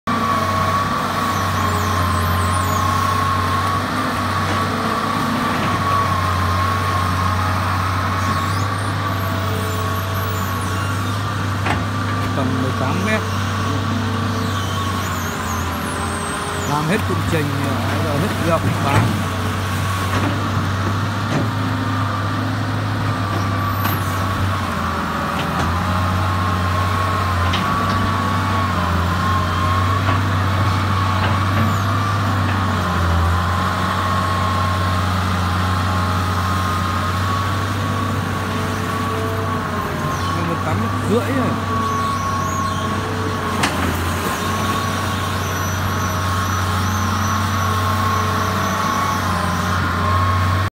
Bán thanh lý máy xúc đào Hitachi ex200-1
Máy hoạt động tốt có bảo hành 3 tháng cho phần bơm và động cơ .